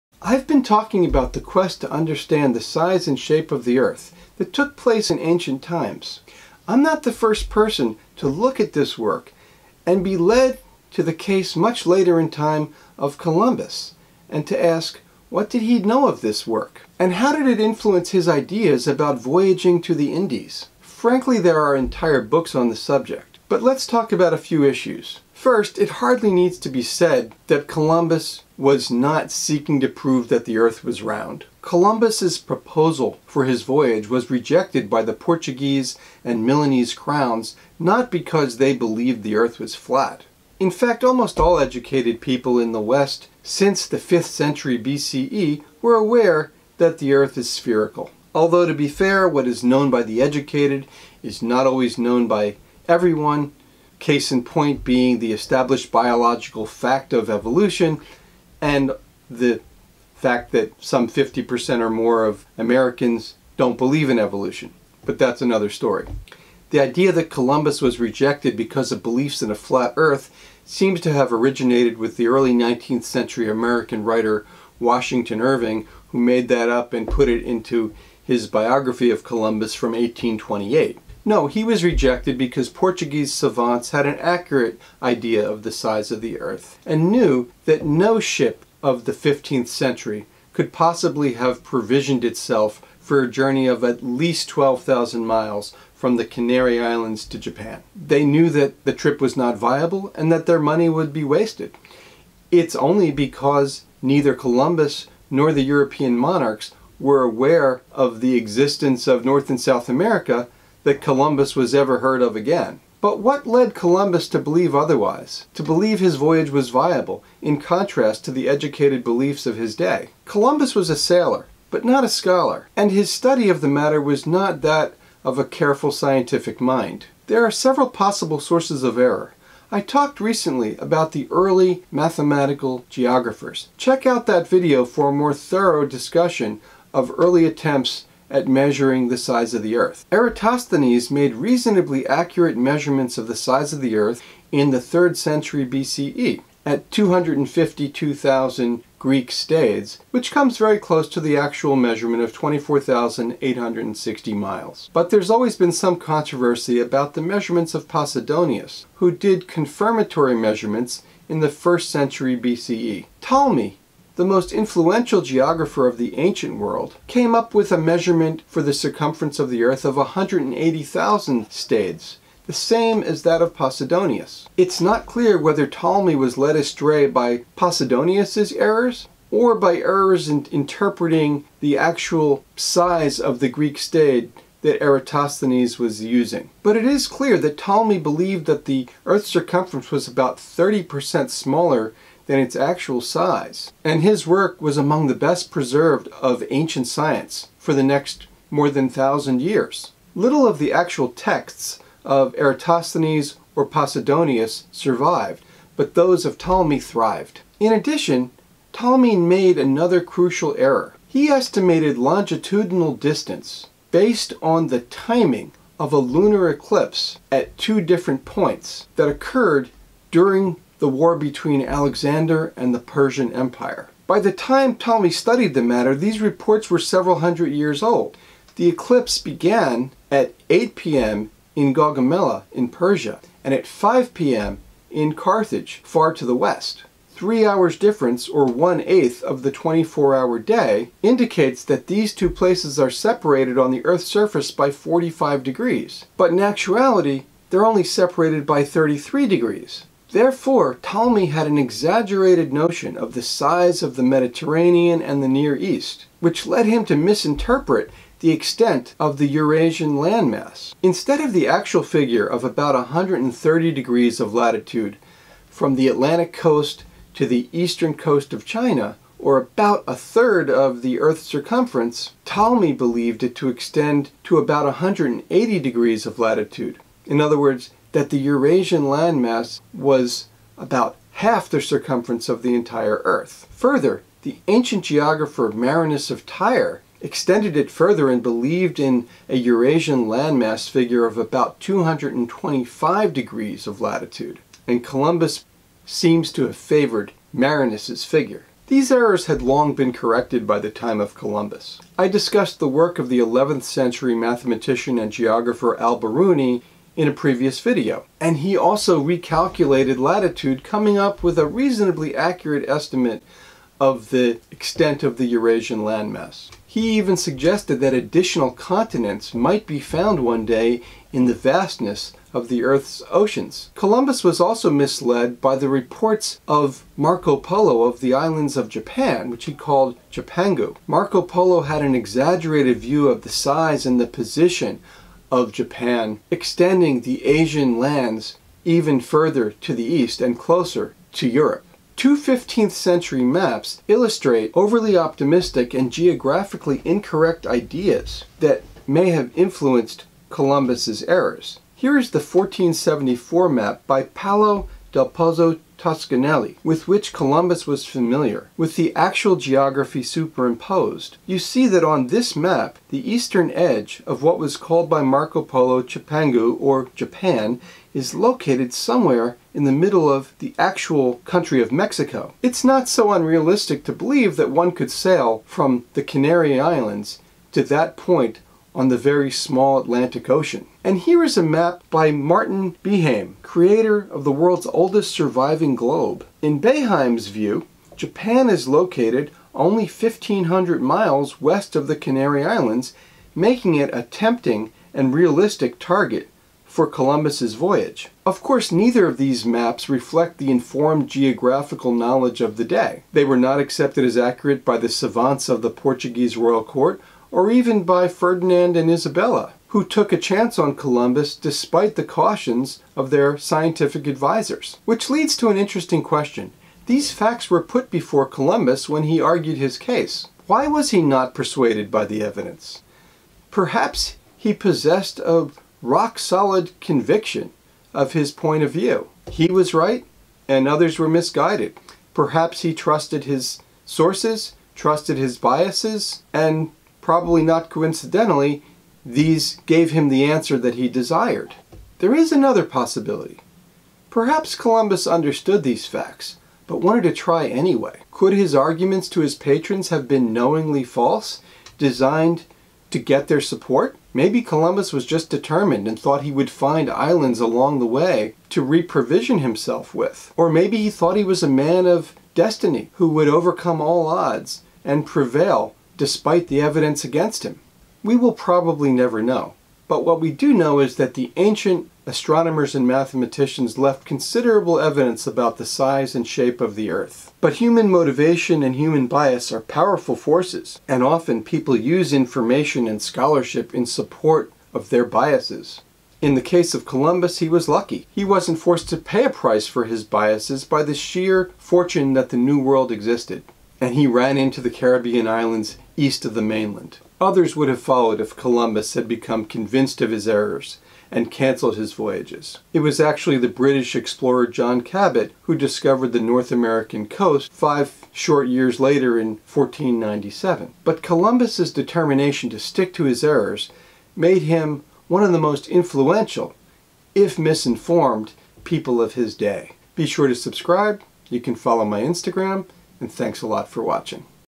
What Sized Earth was Columbus Seeing?
Previous videos have detailed the clever scientific measurements made of the size of the Earth by ancient astronomers and geographers. This video looks at why Columbus disregarded the knowledge of his times, and clung to the belief in a small Earth. Had he known the Earth's true size, he might never had attempted his voyages to "The Indies". Columbus trusted measurements and maps not considered credible by scholars of his day. We examine those maps, and explore Columbus' possible motives and errors.

Image Attributions:

Portrait of Columbus (1556)
Cristofano dell'Altissimo  [Public domain], via Wikimedia Commons

Columbus at the Court of the Catholic Monarchs, Juan Cordero 1850
Museo Nacional de Arte [Public domain], via Wikimedia Commons

Anaxagoras
By Eduard Lebiedzki, after a design by Carl Rahl [Public domain], via Wikimedia Commons

Exploding head
Public domain

Washington Irving 1833 copy bio Columbus

Santa Maria, 15th century ship, 1892 replica
By Possibly by Edward H. Hart. [Public domain], via Wikimedia Commons

1691 Sanson Map of the World, adapted by me
[Public domain], via Wikimedia Commons

Ptolemy with a armillary sphere model, 1476
Justus van Gent and Pedro Berruguete
Wikimedia Commons

Battle of Gaugamela, (1602) Jan Brueghel the Elder
Jan Brueghel the Elder [Public domain], via Wikimedia Commons

Eurasia map
adapted by me with locations of Carthage and Gaugamela

Toscanelli map of the world from 1470 with modern land masses added in 1884
by Bartholomew
By Bartholomew, J. G. [Public domain], via Wikimedia Commons

Martin Behaim image of globe 1492
By MartinBehaim1492.jpg: Encyclopedia Larousse illustree - 1898derivative work: PawełMM (MartinBehaim1492.jpg) [Public domain], via Wikimedia Commons

Columbus Before the Queen (1843) by Emanuel Leutze
Emanuel Leutze [Public domain], via Wikimedia Commons

Painting of John Cabot in traditional Venetian garb by Giustino Menescardi (1762)
By Giustino Menescardi [Public domain], via Wikimedia Commons